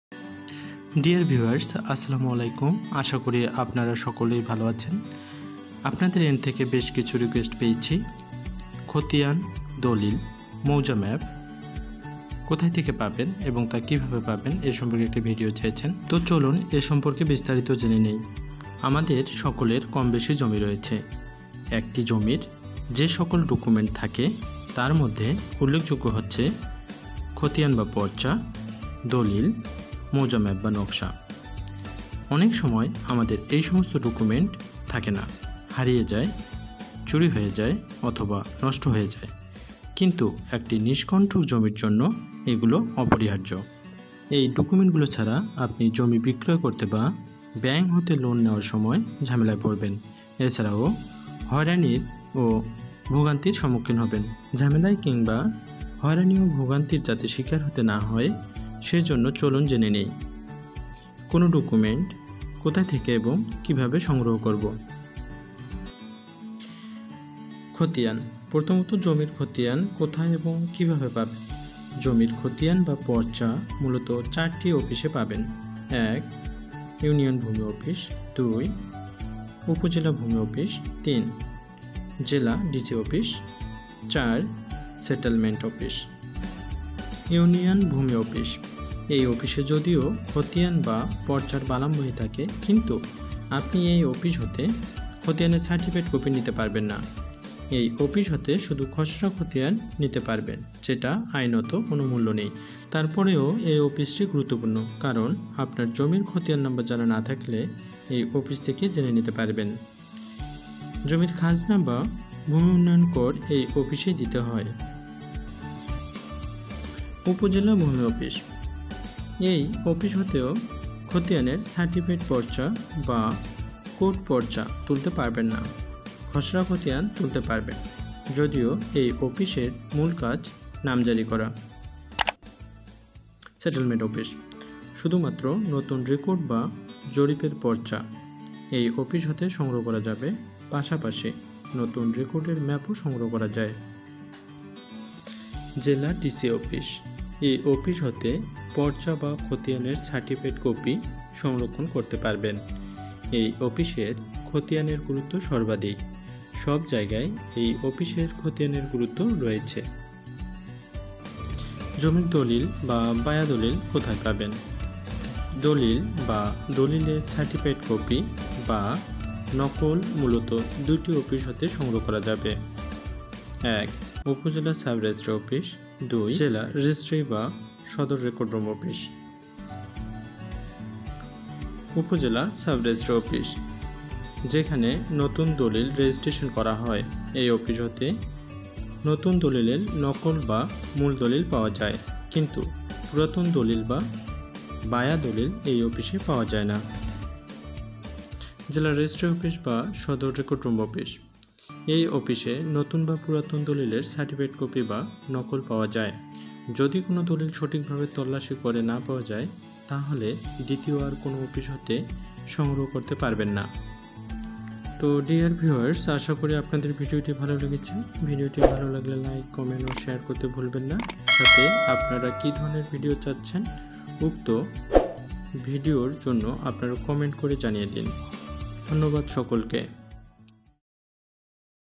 জমির দলিল খতিয়ান মৌজা ম্যাপ হারিয়ে গেলে বা নষ্ট হলে কিভাবে এবং কোথায় পাবেন?
আপনার জমির দলিল, খতিয়ান, মৌজা ম্যাপ হারিয়ে গেলে কিভাবে এবং কোথায় পাবেন?